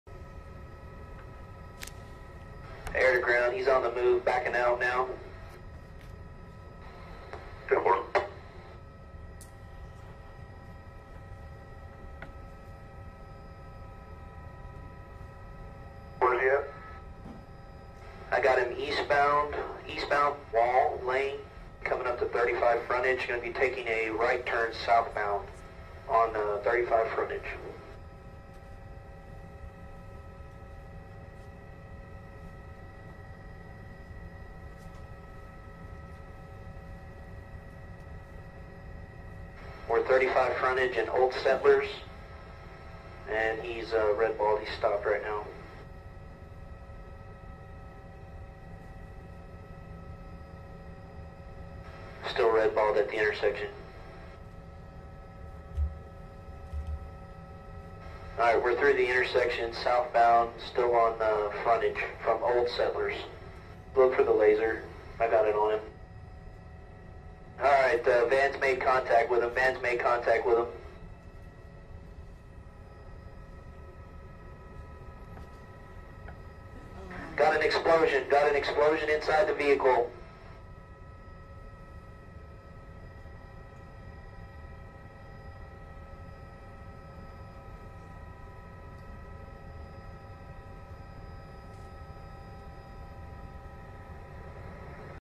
New footage shows shocking moment Austin bomber blows up his car
Aerial surveillance footage released by the Texas Department of Public Safety shows the moment Austin serial bomber Mark Anthony Conditt blows up his car. A SWAT team is seen approaching the car before an explosion goes off.